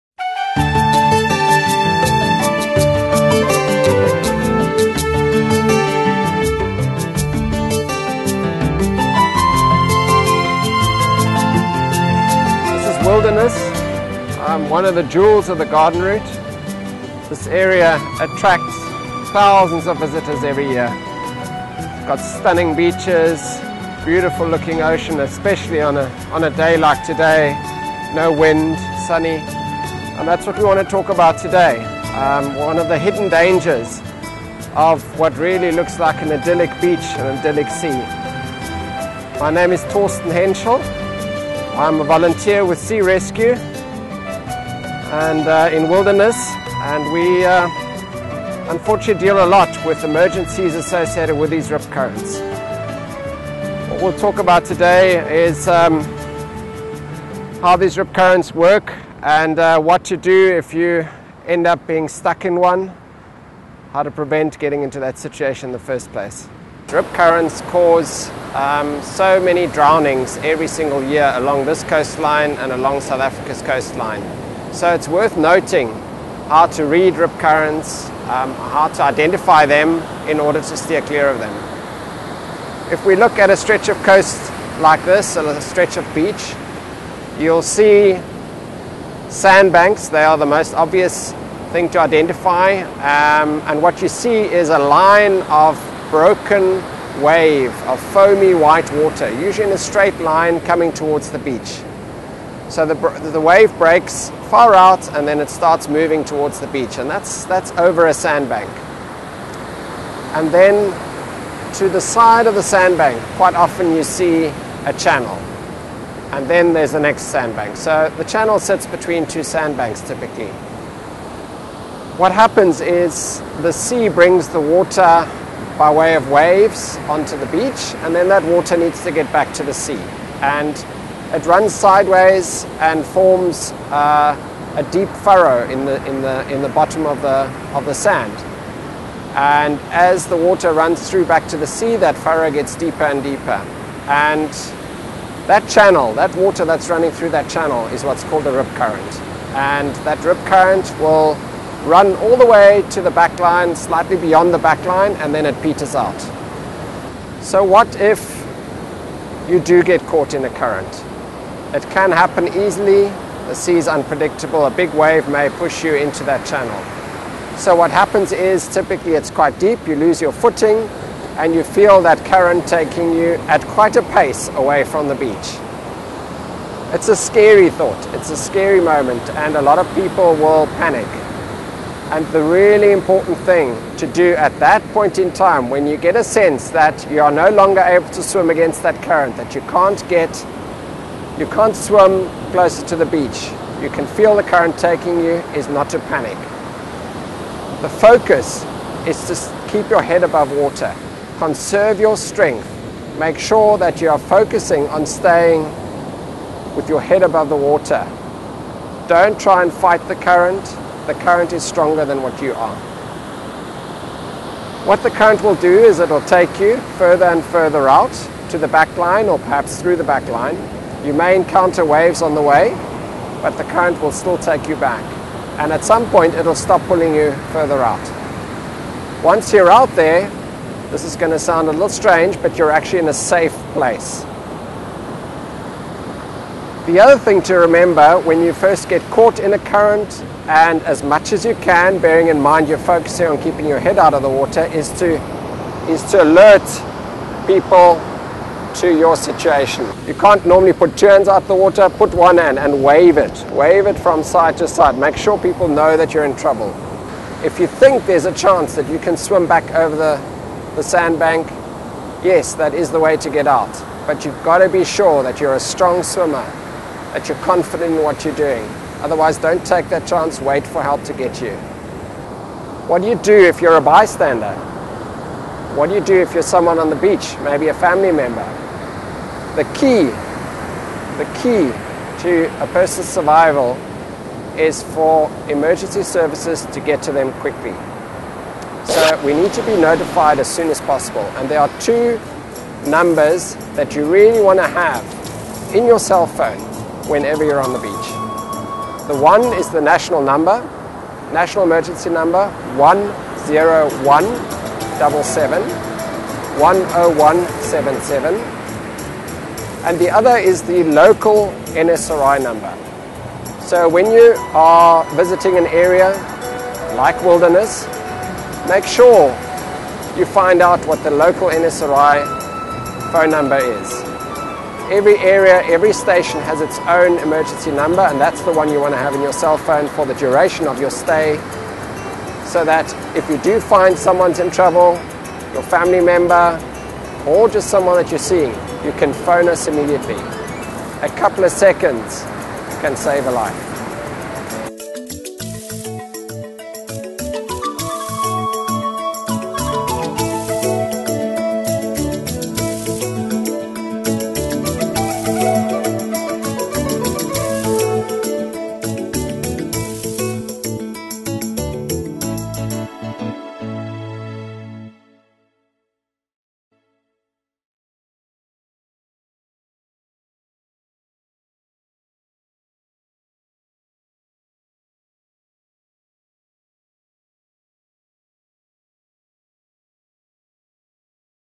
Rip Currents
This six minute Sea Rescue video shows how to identify rip currents, what to do if you are caught in a rip and who to call if you see someone in difficulty on the South African coast.